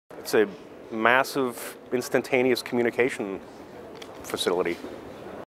Internet Future in a Nutshell? IETF 2015: Dan Harkins
Dan Harkins, Scientist at Aruba Network, shares his answer to the question “Using just a few words, in just five seconds or less, describe the future of the Internet.” at Internet Engineering Task Force meeting 92, which took place in March 2015 in Dallas, Texas. Recorded by the Imagining the Internet Center, an initiative of Elon University, North Carolina, USA.